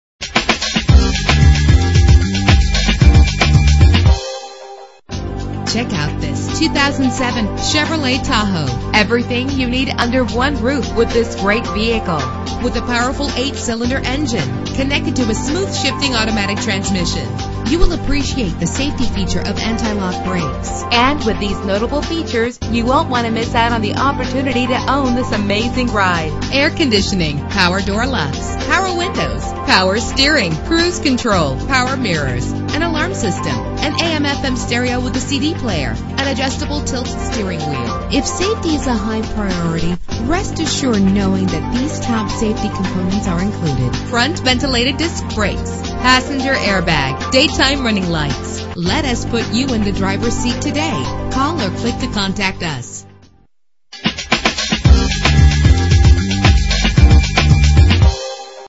2007 Chevrolet Tahoe - Little Rock AR
Year: 2007
Make: Chevrolet
Model: Tahoe
Trim: LT
Engine: 5.3 liter 8 cylinder 16 valve
Transmission: 4-Speed Automatic
Color: Silver
Mileage: 61512
Address:
10825 Colonial Glenn Road